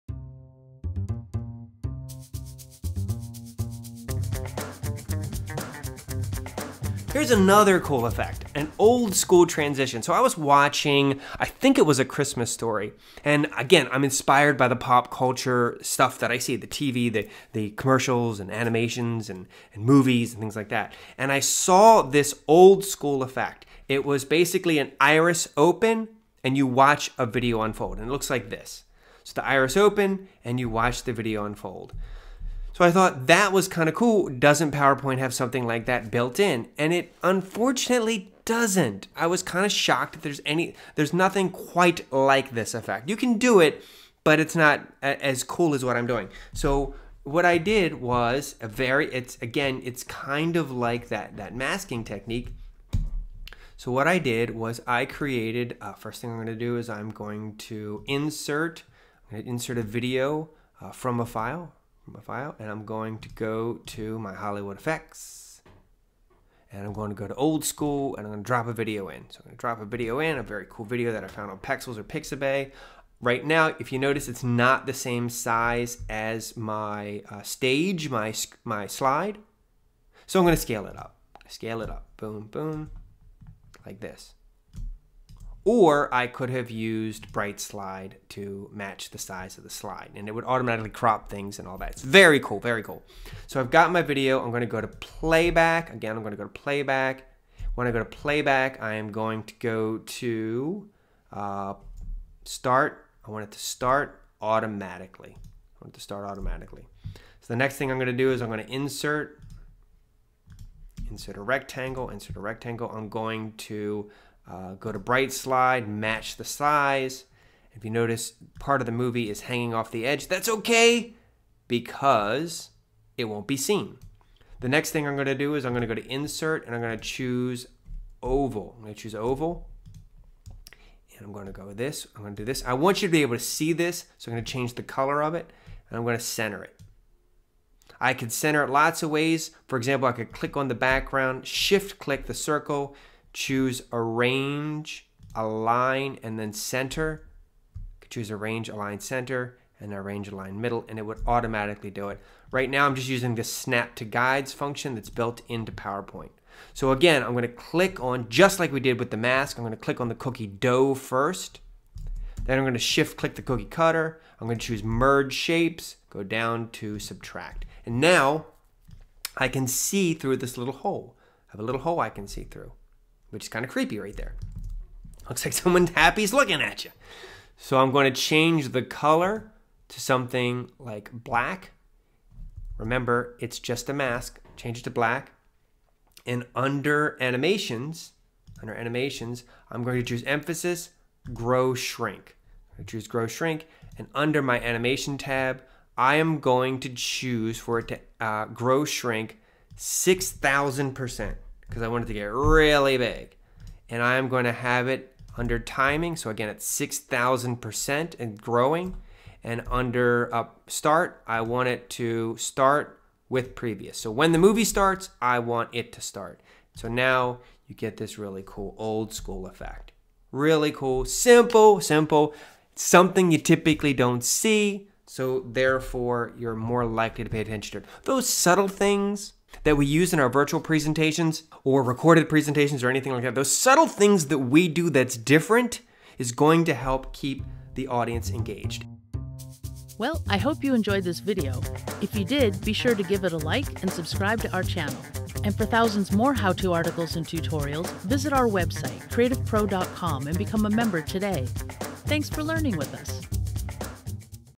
PowerPoint: How to Create an Iris Transition Effect (Video Tutorial)
In this PowerPoint video, Mike Parkinson shows how to build an opening iris effect into a presentation. By creating and merging shapes, he uses the built-in animations to create a cinematic transition.

This tutorial was taken from Mike’s “Hollywood FX for a Virtual Audience” session at our 2021 PowerPoint Presentation Design Deep Dive.